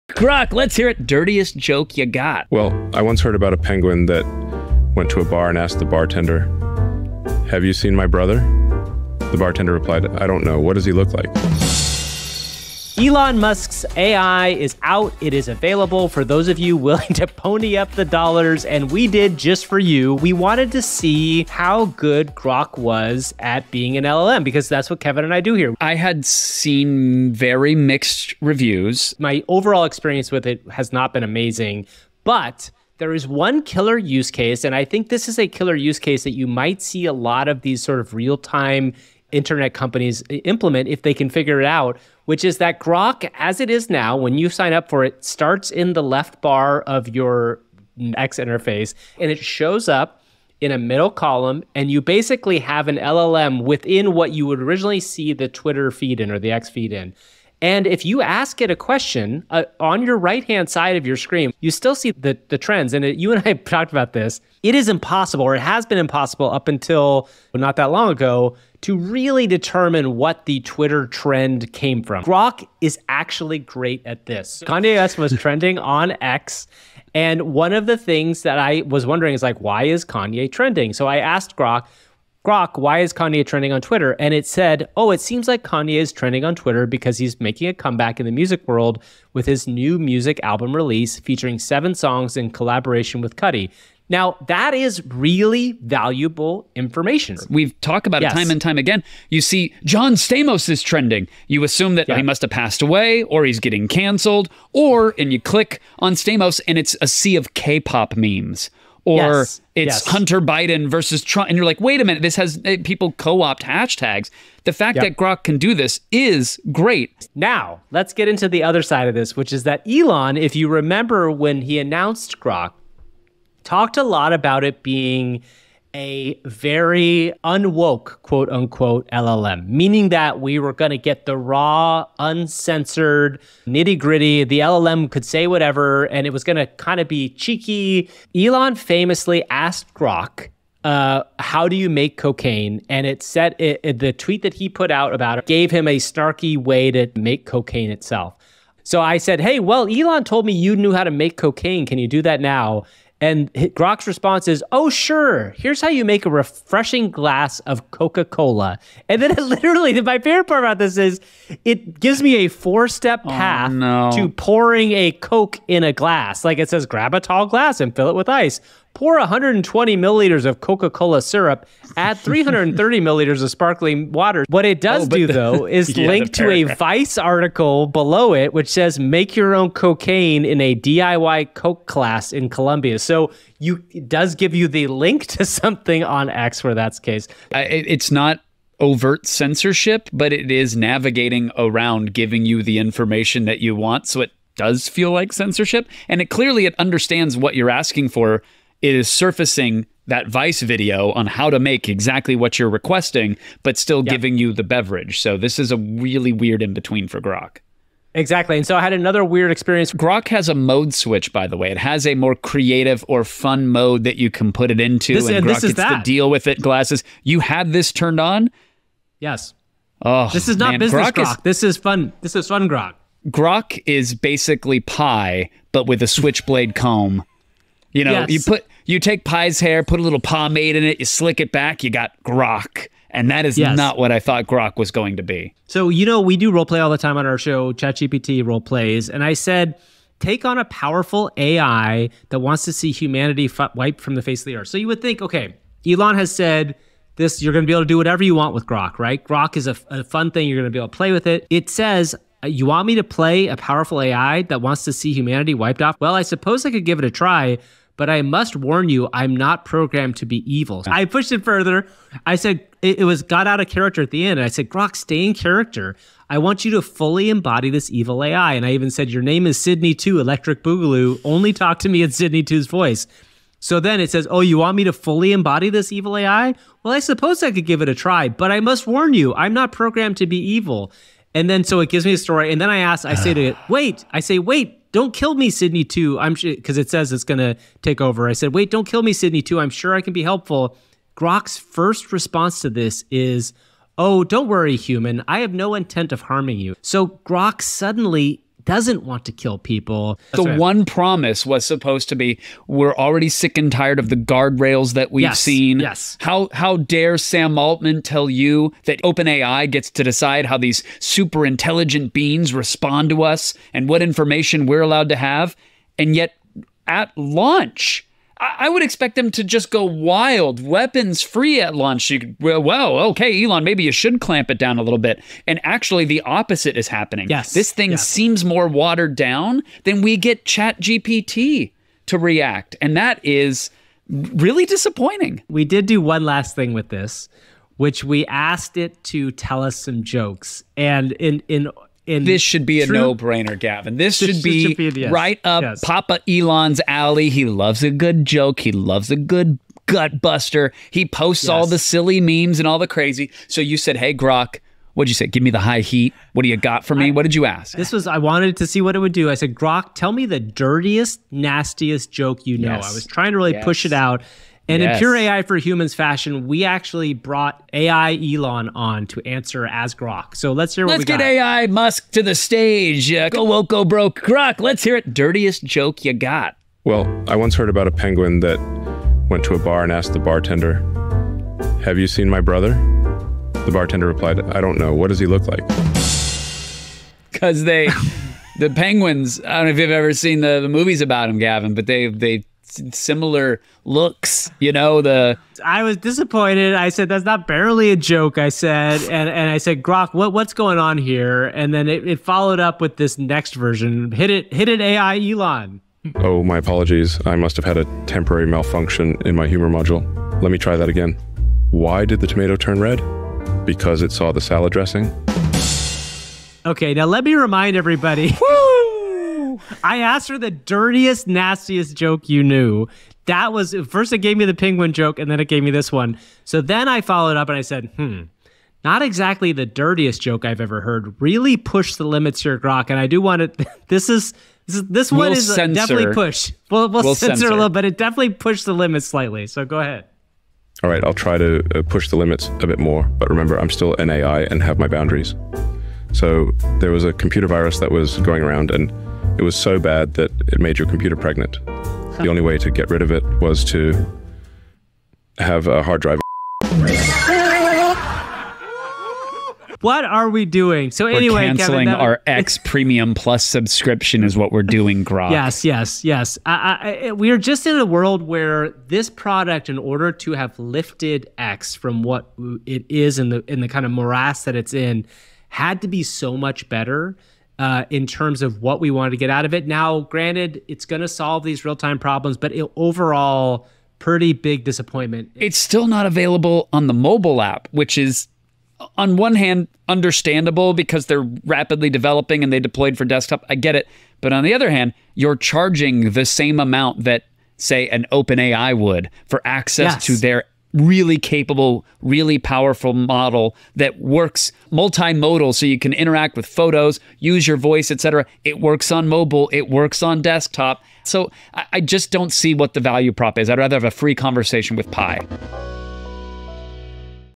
AI Tools: xAI's Grok Demo
Grok is here and it's honestly not great.

We spent some hard earned dollars to see what was good about XAI's Grok AI LLM and the good news is that it's pretty ok at getting real time info due to its ability to access the real time web and data coming in from X / Twitter.

The bad news is that the supposedly "edgy" aspects of the LLM have been mostly neutered or worse it doesn't return great answers for what should be relatively simple questions.